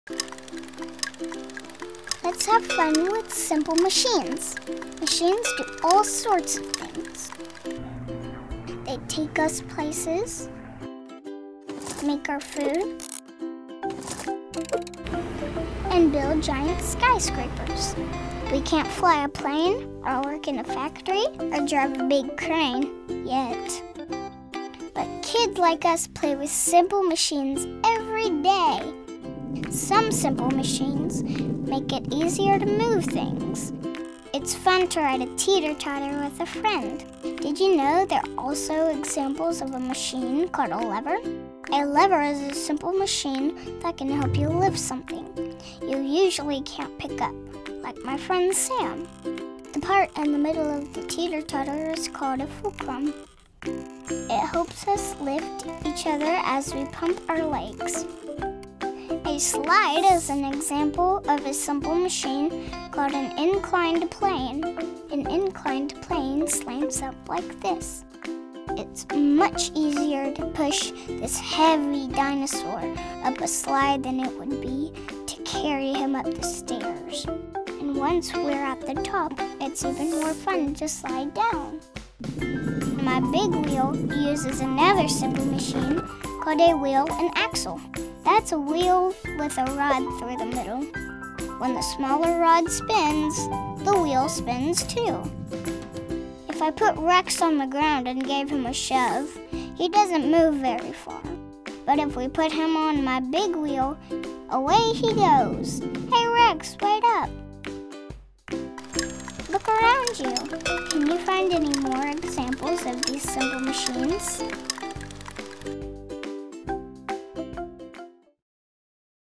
Let’s Have Fun With Simple Machines!
This nutshell was created for Kentucky Educational Television as part of an early childhood education series and explores what simple machines can do and how they work. To see all the instructional support resources for this video and others like it,